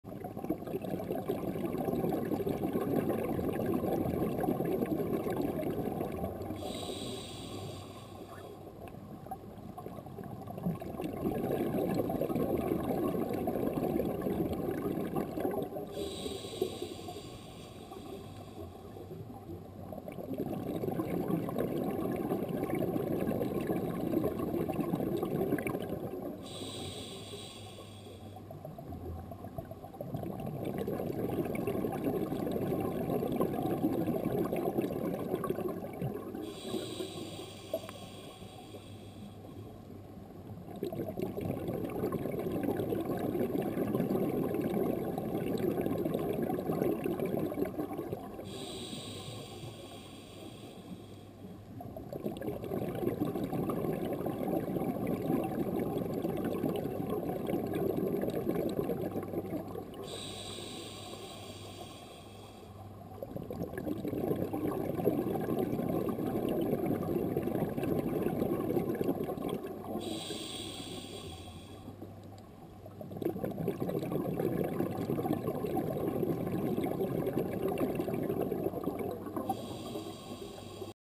Conch hunting Garden Eel
Filmed at Chain Wall Reef in Belize on 11-5-13.  Features Strombus gigas and Heteroconger.